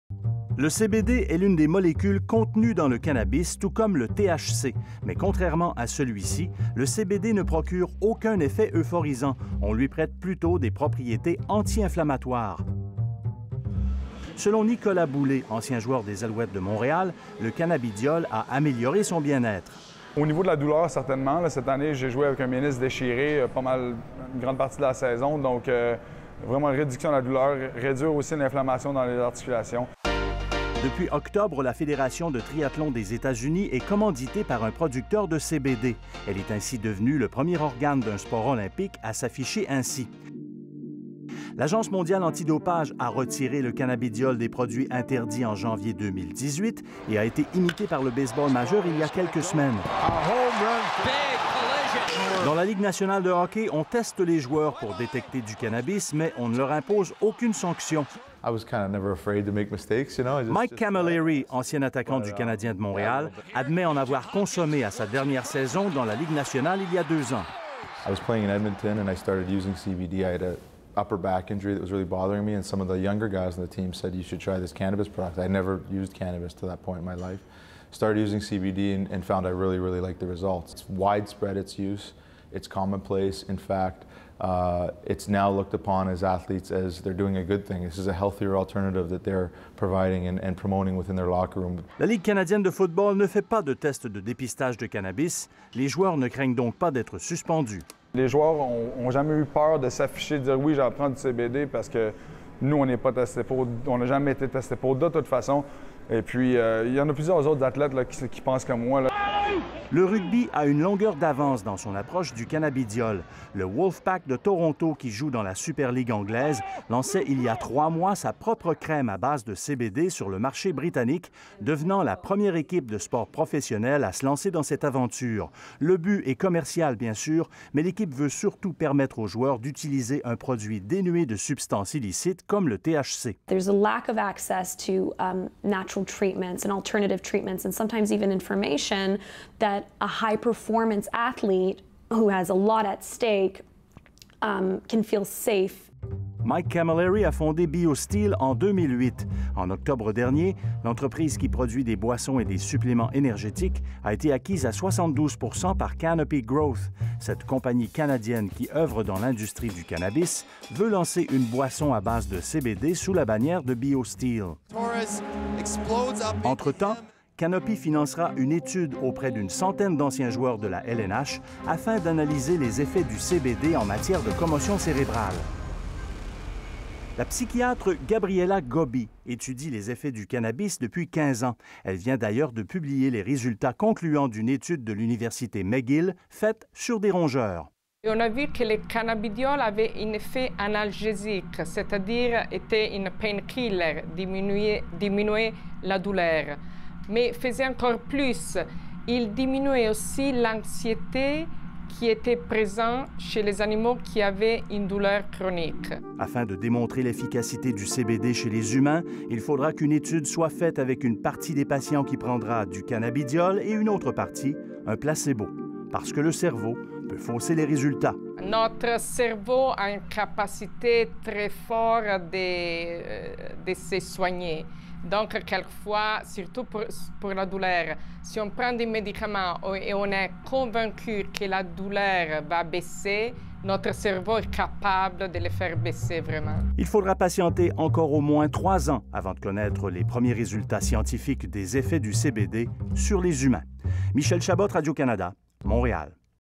Cannabidiol : de plus en plus populaire chez des athlètes
L'utilisation du cannabis et de ses dérivés se banalise. Dans le monde du sport, de plus en plus d'athlètes délaissent les opioïdes pour soigner leurs douleurs au profit d'un sous-produit du cannabis, le CBD. Le reportage de Michel Chabot